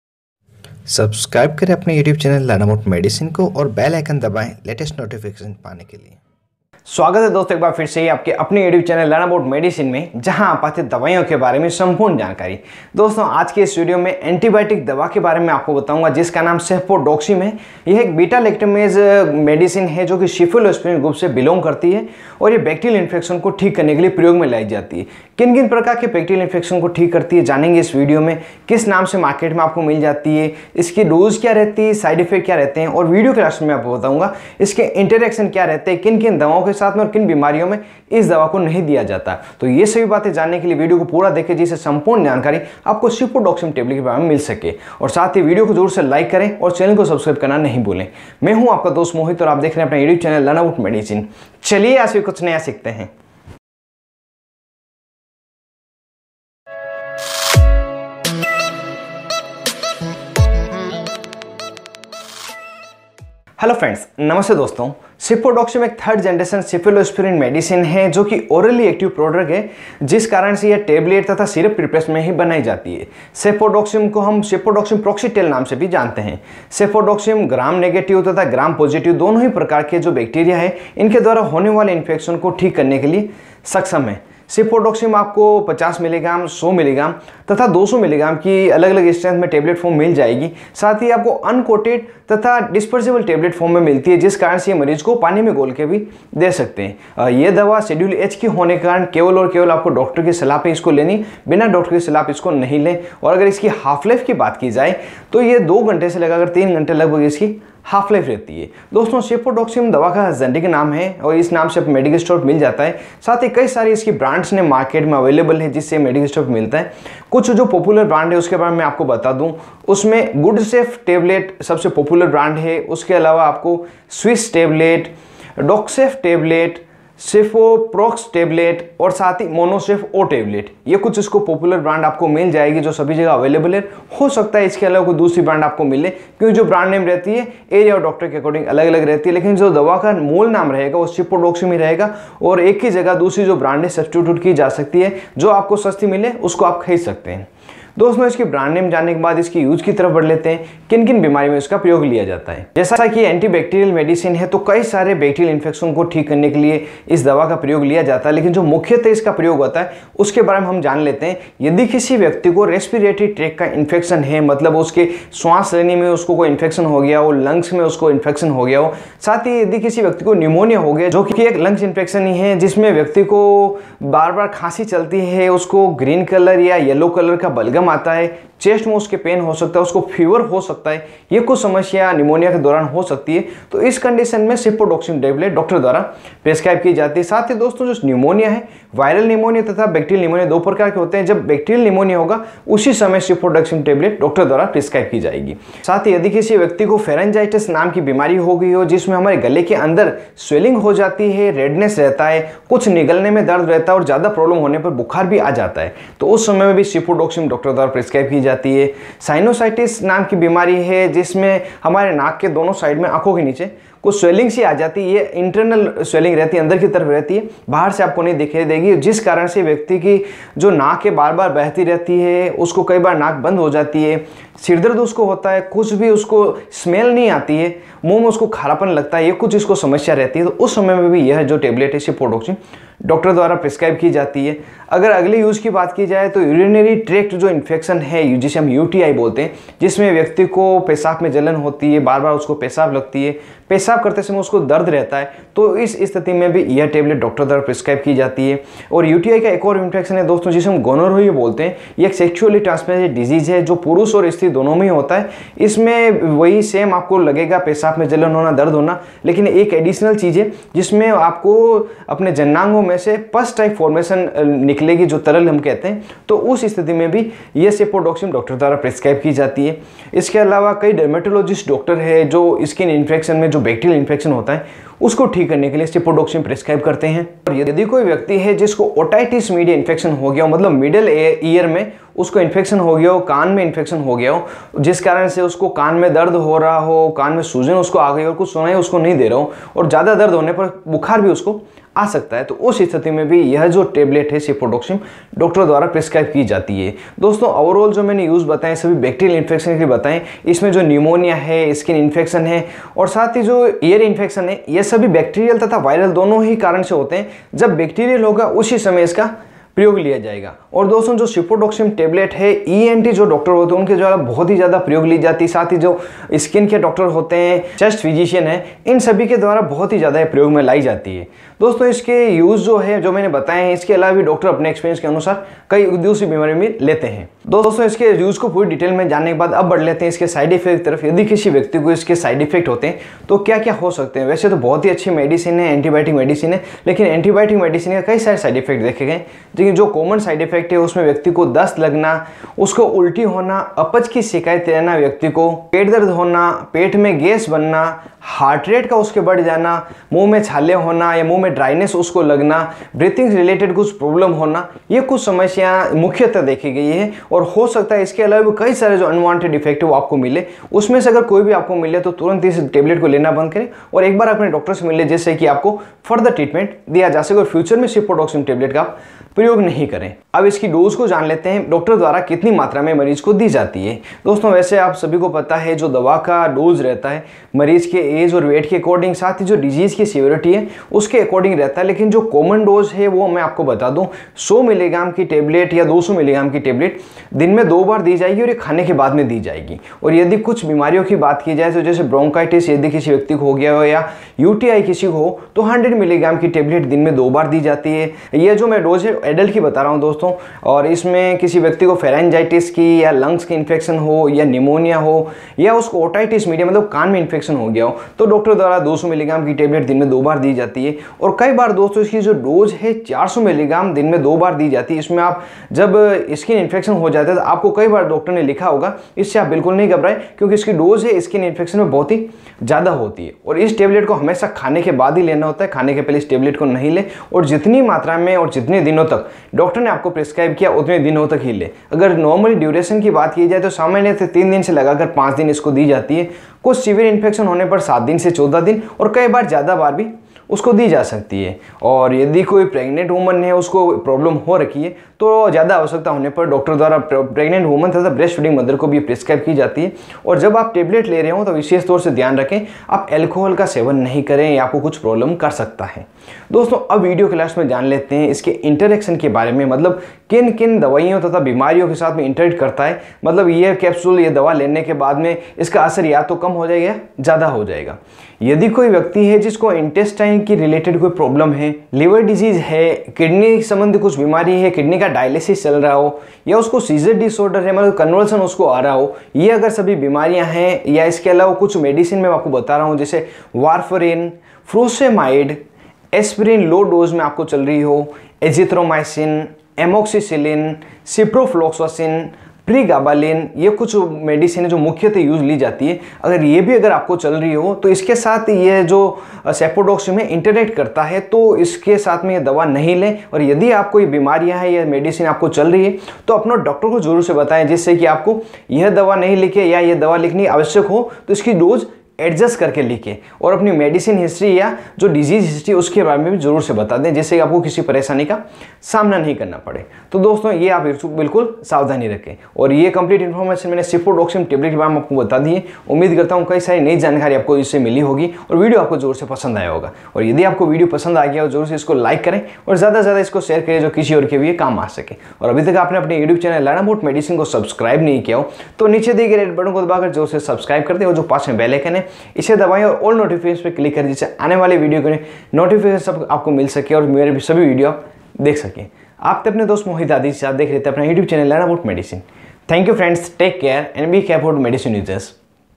Cefpodoxime tablet | Cefpodoxime proxetil tablet | Cefpodoxime 200 | Cepodem 200
Cefpodoxime is beta lactam antibiotic medicine belongs to cephalosporin class. cefpodoxime is 3rd generation cephalosporin.
cefpodoxime is orally active prodrug.
cefpodoxime tablet also named as cefpodoxime proxetil tablet.
cefpodoxime tablet strength is
• cefpodoxime 50 mg tablet
• cefpodoxime 100 mg tablet
• cefpodoxime 200 mg tablet
And it is available in dispersible tablet.
cefpodoxime tablet popular brand name are
1. Gudcef tablet | gudcef 200
2. Swich tablet
3. Doxcef tablet | doxcef 200
4. Cefoprox tablet
5. Monocef O
6. Cepodem 200 tablet with many other
cefpodoxime proxetil tablet uses to treat
• Respiratory tract Infection
• Lungs infection
• Bronchitis
• Pneumonia
• Pharyngitis
• Sinusitis
• UTI
• Gonorrhoea
• Skin infection
• Otitis media infection
cefpodoxime tablet side effects are
• Diarrhoea
• Loose motion
• Abdominal pain
• Vomiting
• Indignation
• Gastritis
• mouth ulcer
• Dryness of mouth
• Fast heart rate
• Edema
cefpodoxime tablet interact with many disease and medicine such as
Diseases: -
• Intestine disease
• Liver disease
• Kidney problem
• Dialysis
• Seizure disorder
Medicines
• Warfarin
• Furosemide
• Aspirin
• Azithromycin
• Amoxycillin
• Ciprofloxacin
• Pregabalin etc..
we also discuss about cefpodoxime tablet |
• strength
• brand name
• uses in detail
• side effects
• dosage
• interaction and many more in hindi.